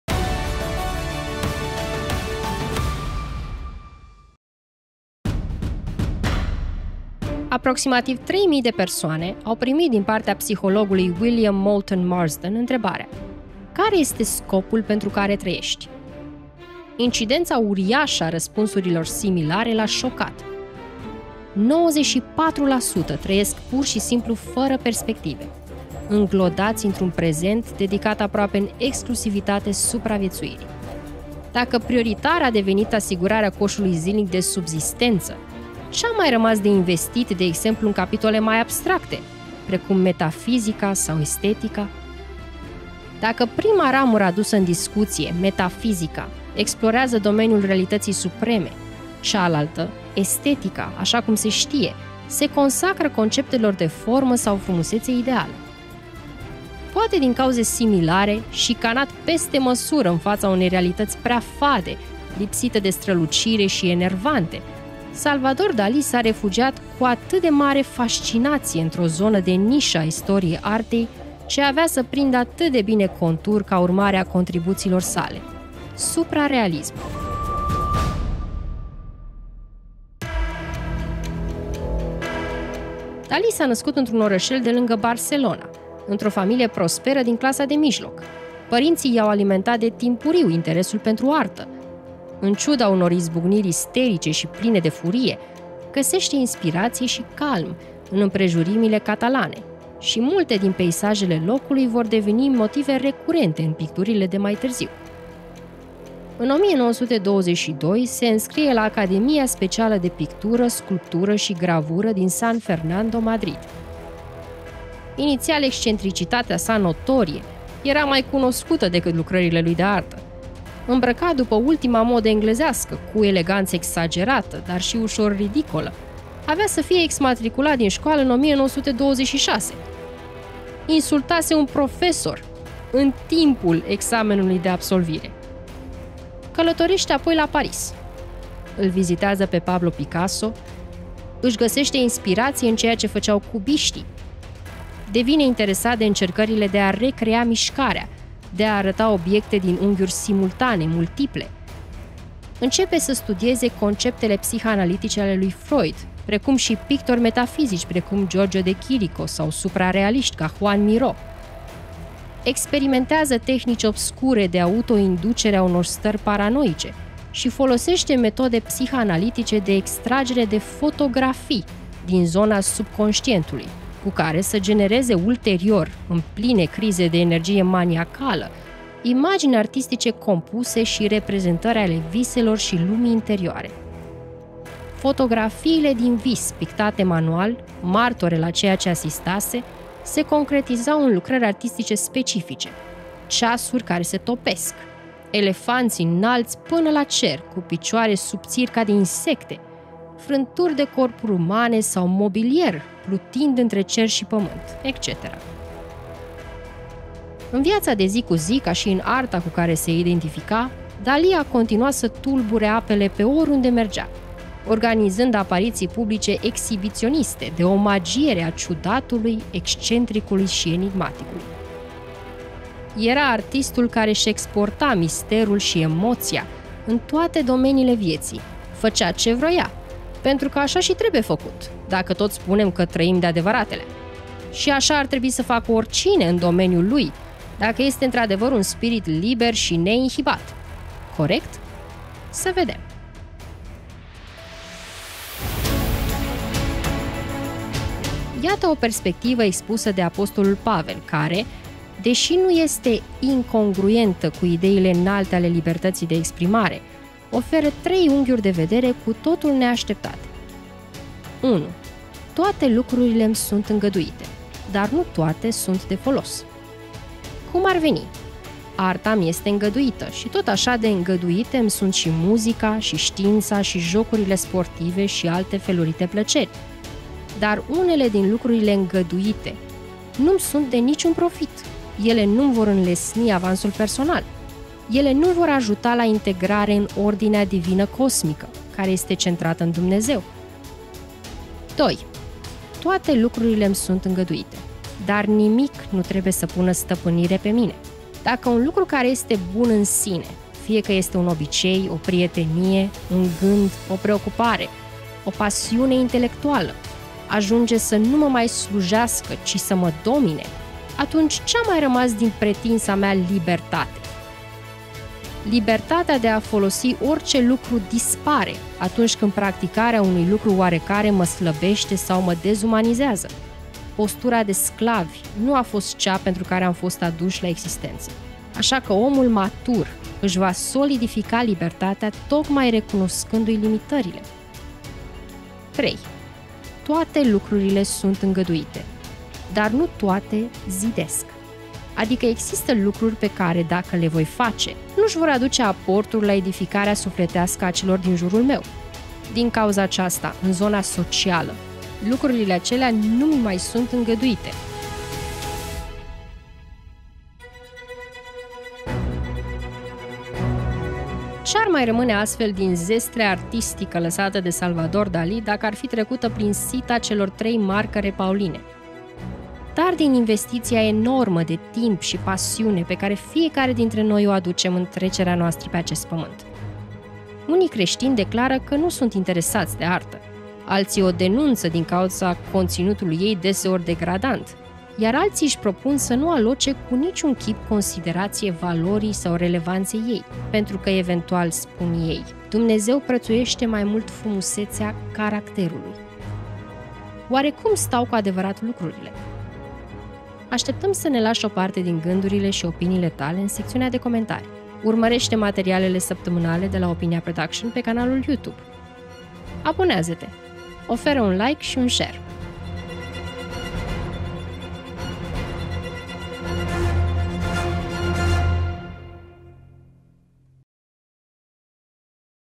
SALVADOR DALI. DE CE SE TOPESC CEASURILE? BIBLIA DESPRE ARTĂ
Salvador Dali. Libertate si pictura. Aici afli cat i-ar fi ingradit Biblia libertatea de expresie in picturile/arta lui Salvador Dali. Te poti abona aici

00:00 Scopul vietii
01:19 Dali. Omul. Artistul. Suprarealismul
04:04 Biblia si libertatea in arta

 Aproximativ 3000 de persoane au primit din partea psihologului William Moulton Marston întrebarea: „Care este scopul pentru care traiesti?”
 Incidența uriașă a răspunsurilor similare l-a șocat: 94% trăiesc pur si simplu fără perspective, înglodați într-un prezent dedicat aproape în exclusivitate supraviețuirii!
 Poate din cauze similare, șicanat peste măsură în fața unei realități prea fade, lipsite de strălucire și enervante, Salvador Dali s-a refugiat cu atât de mare fascinație într-o zonă de nișă a istoriei artei ce avea să prindă atât de bine contur ca urmare a contribuțiilor sale: suprarealismul!
 În 1922 se înscrie la Academia specială de pictură, sculptură și gravură din San Fernando, Madrid. Inițial excentricitatea sa notorie era mai cunoscută decât lucrările lui de artă. Îmbrăcat după ultima modă englezească, cu eleganță exagerată dar și ușor ridicolă, avea să fie exmatriculat din scoală în 1926. Insultase un profesor în timpul examenului de absolvire!
 Călătorește apoi la Paris. Îl vizitează pe Pablo Picasso. Își găsește inspirație în ceea ce făceau cubiștii. Devine interesat de încercările de a recrea mișcarea, de a arăta obiecte din unghiuri simultane, multiple.
 Experimentează tehnici obscure de  auto-inducere a unor stări paranoice și folosește metode psihanalitice de extragere de „fotografii” din zona subconștientului cu care să genereze ulterior - în pline crize de energie maniacală - imagini artistice compuse și reprezentări ale viselor și lumii interioare.
 În viața de zi cu zi, ca și în arta cu care se identifica, Dalí a continuat să tulbure apele pe oriunde mergea, organizând apariții publice exibiționiste de omagiere a ciudatului, excentricului și enigmaticului.
 Era artistul care-și exporta misterul și emoția în toate domeniile vieții! Făcea ce vroia, pentru că așa și trebuie făcut - dacă tot spunem că trăim de-adevăratelea..! Și așa ar trebui să facă oricine, în domeniul lui, dacă este într-adevăr un spirit liber și neinhibat, corect..? Să vedem.
 Iată o perspectivă expusă de apostolul Pavel care - deși nu este incongruentă cu ideile înalte ale libertății de exprimare - oferă trei unghiuri de vedere cu totul neașteptate.
 Unu: "Toate  lucrurile imi sunt îngăduite, dar nu toate sunt de folos" (1 Cor 10:23pp)
 Cum ar veni - arta îmi este îngăduită; și tot așa de îngăduite îmi  sunt și muzica, și știința și jocurile sportive și alte felurite plăceri. Dar unele din lucrurile îngăduite nu-mi  sunt de nici un profit; ele nu-mi  vor înlesni avansul personal.
 Doi: "Toate lucrurile imi sunt îngăduite, dar nimic nu trebuie să pună stăpânire pe mine"( 1 Cor 6:12)
 Dacă un lucru care este bun în sine - fie că este un obicei, o prietenie, un gând,   o preocupare, o pasiune intelectuală - ajunge sa nu mă mai slujească, ci să mă domine, atunci ce a mai rămas din pretinsa mea "libertate" ?
 Trei: "Toate  lucrurile sunt îngăduite, dar nu toate zidesc" (1 Cor 10:23up)
 Adică există lucruri pe care, daca le voi face, nu își vor aduce aportul la edificarea sufletească a celor din jurul meu. Din cauza aceasta, în zona socială, lucrurile acelea nu-mi mai sunt îngăduite.
 Ce-ar mai ramane astfel din zestrea artistică lăsată de Salvador Dali dacă ar fi trecută prin sita celor trei marcăre pauline...?
 Unii creștini declara ca nu sunt interesati de arta. Alții o denunță din cauza conținutului ei deseori degradant. Iar altii își propun să nu aloce cu nici un chip considerație valorii sau relevanței ei.  Pentru că eventual - spun ei - Dumnezeu prețuiește mai mult frumusețea... caracterului!
 Oare cum stau cu adevărat lucrurile?
 Așteptăm să ne lași o parte din gîndurile și opiniile tale în secțiunea de comentarii.
 Urmărește materialele săptămânale de la Opinia Production pe canalul Youtube.

Opinia Production este un storyteller care explorează lumea şi experiența umană din perspectivă creștină. Explorează și tu cu noi!